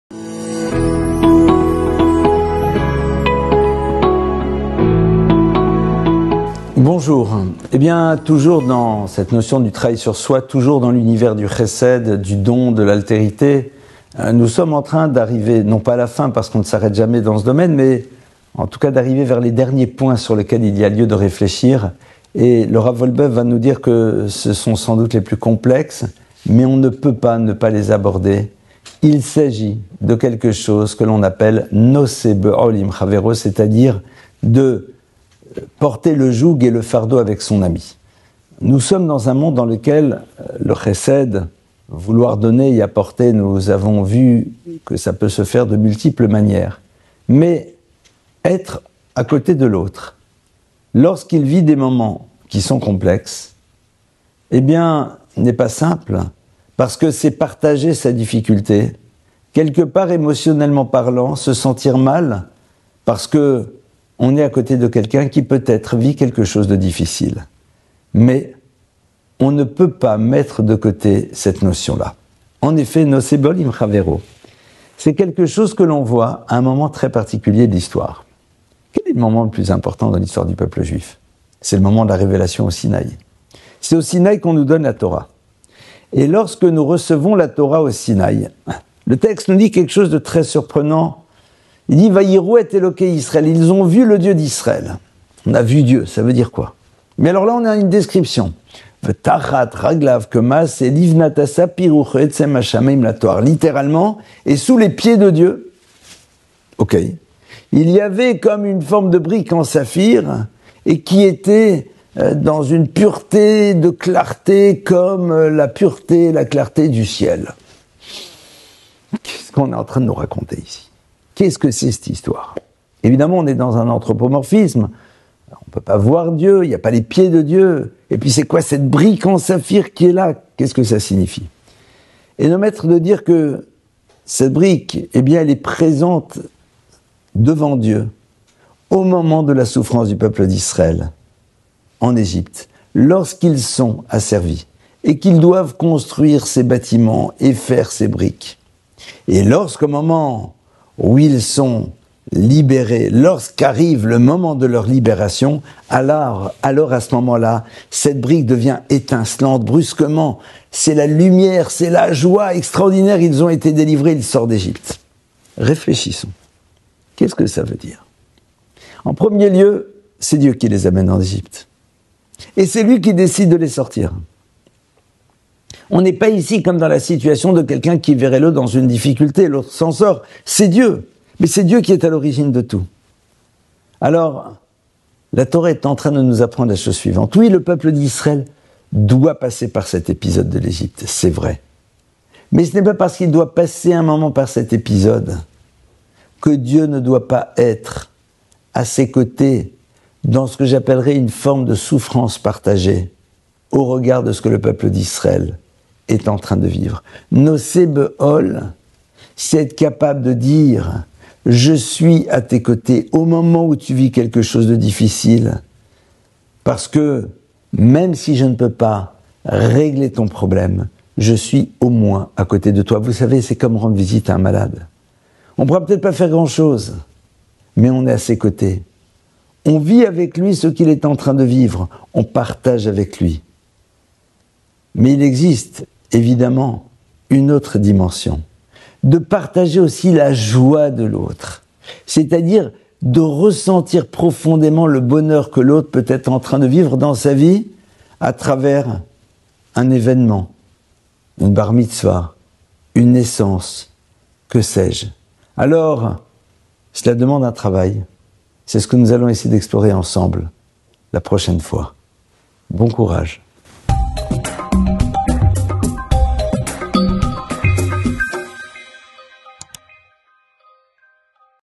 #33 - Je suis à tes côtés - Rav Elie Lemmel
Devenir une meilleure version de soi-même... Rav Elie Lemmel nous offre un enseignement concret à appliquer dans notre quotidien.